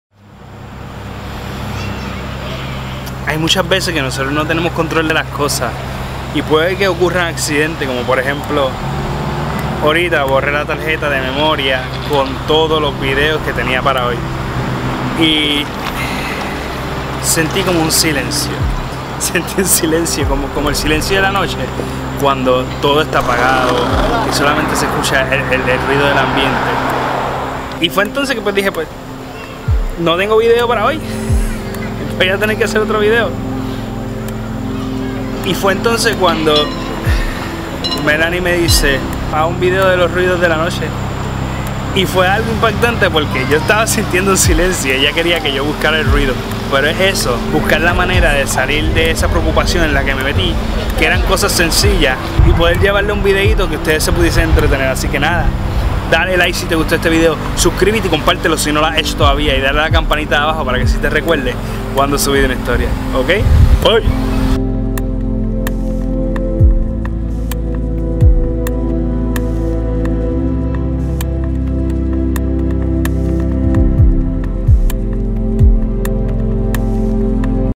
Del silencio pase al ruido de la noche
365 Episode 48
No hay cosa peor que tu tener un deadline para hacer y subir un video y que sin querer borres la tarjeta de memoria pensando que habías pasado todo a la computadora cuando no era cierto. Borre la tarjeta con un video programado, grabado y todo. Sentí que de repente todo estaba en silencio.